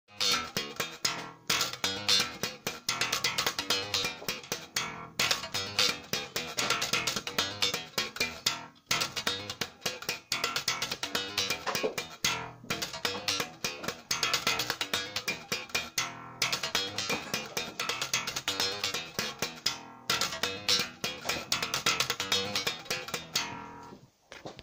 Bass line for my new tune on '09 Fender Jazz Classic Custom Shop
This is part of my bass line, but you can use it like exercise...